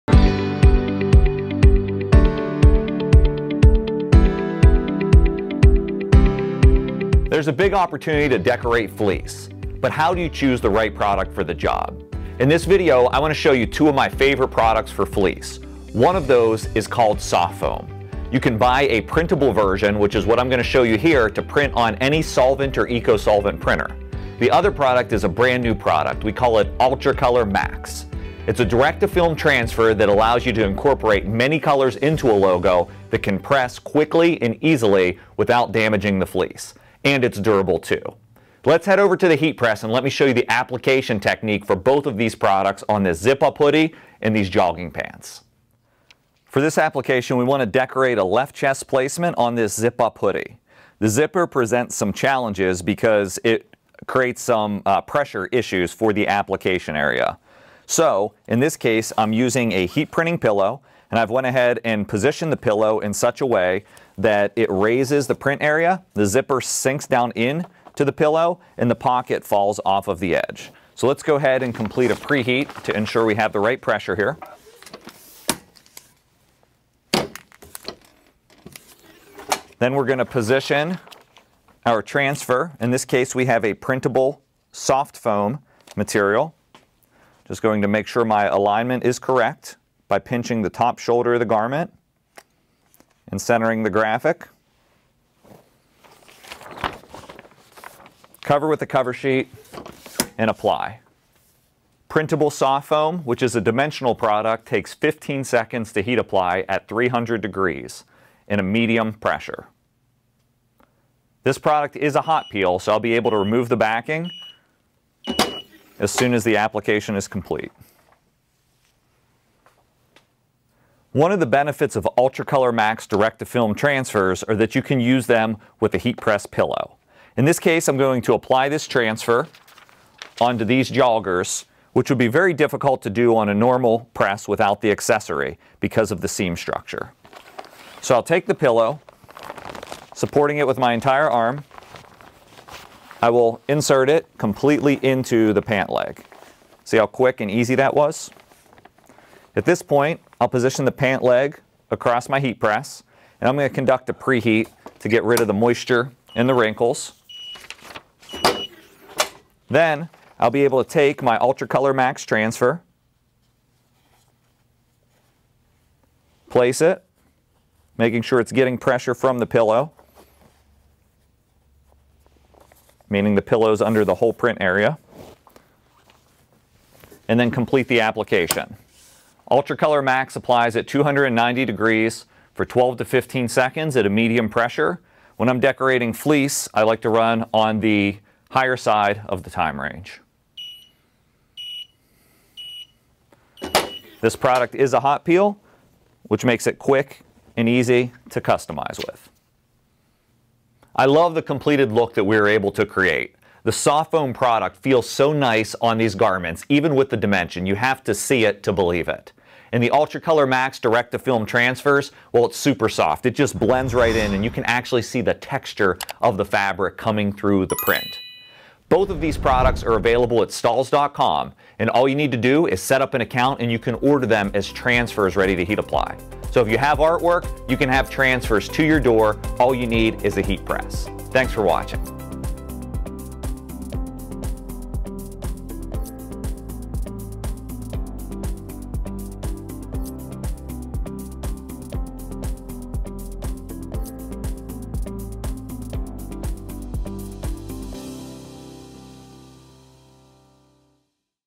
The Best Heat Transfer Options for Fleece Apparel
Are you looking to heat press custom designs onto fleece apparel, but not sure which heat transfers you should be using? Well, look no further because Josh is here to show you how CAD-COLOR® Soft Foam Printable HTV and our brand new UltraColor™ MAX Direct to Film transfers can be the perfect solution when decorating on fleece. These two heat transfer materials offer you unique features that are not found in other types of transfers, such as dimension, a soft feel, and even full color options as well.

To order either of the products featured in this video simply click the links featured below: